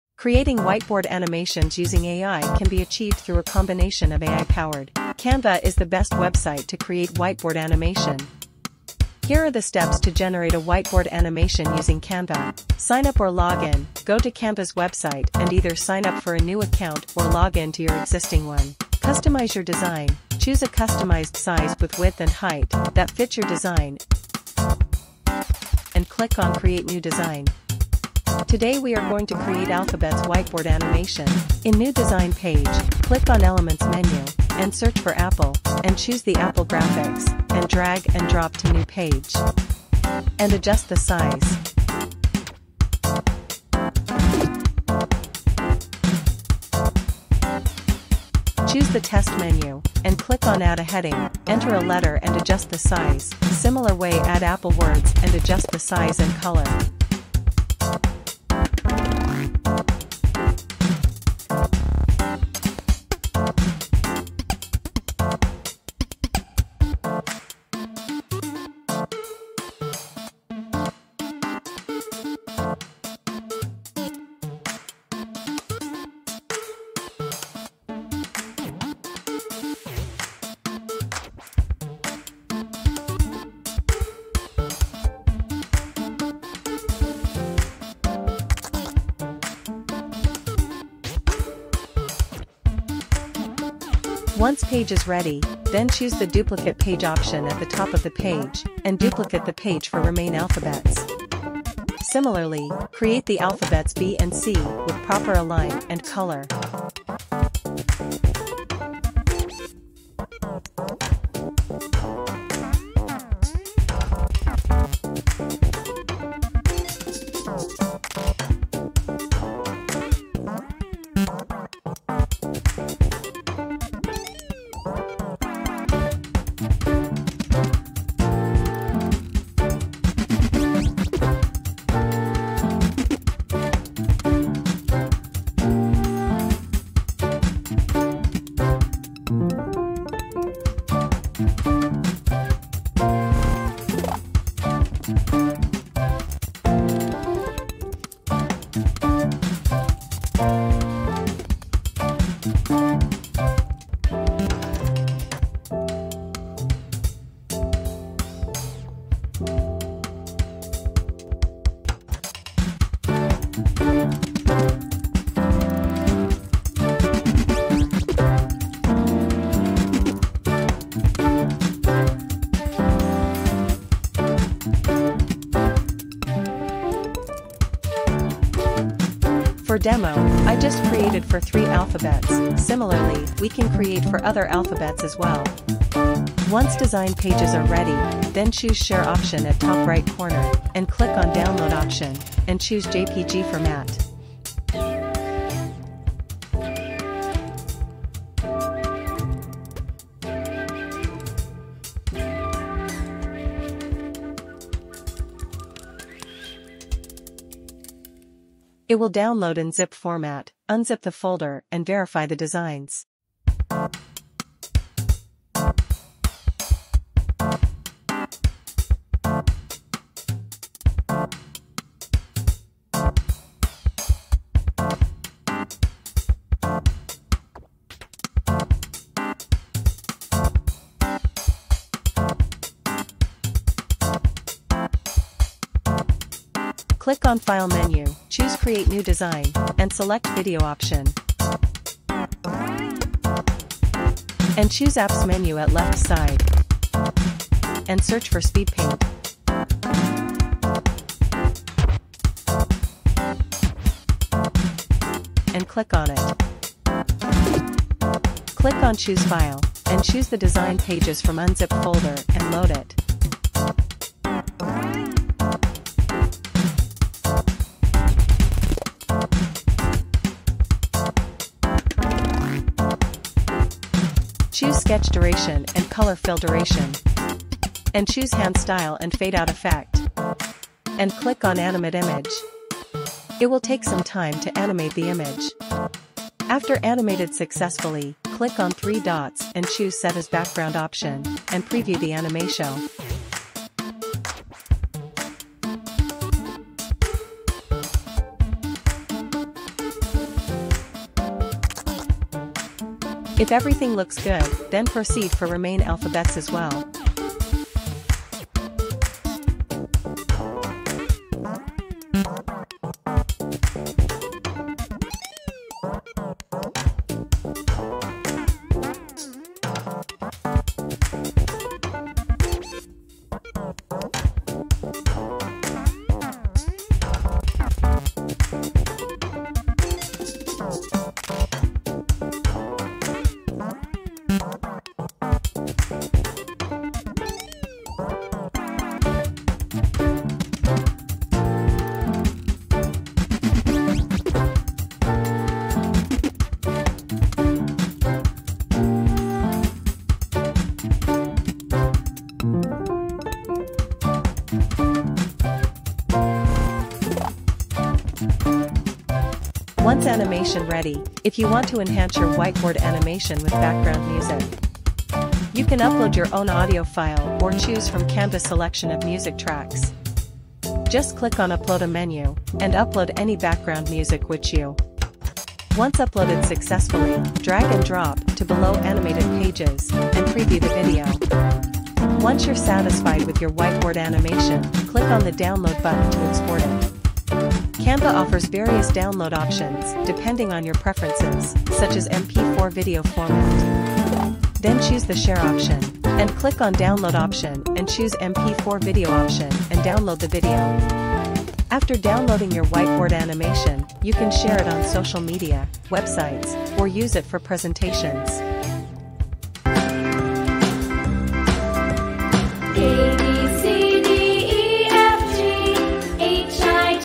White Board Animation | AI
Sign Up or Log In: Go to Canva's website and either sign up for a new account or log in to your existing one.

Choose a Template: Once you're logged in, navigate to the "Templates" section and search for "whiteboard animation." Canva provides various templates to choose from.

Customize the Template: Select a template that fits your needs, and then customize it by adding text, images, icons, and other elements to the whiteboard canvas. Canva offers a user-friendly drag-and-drop interface for easy editing.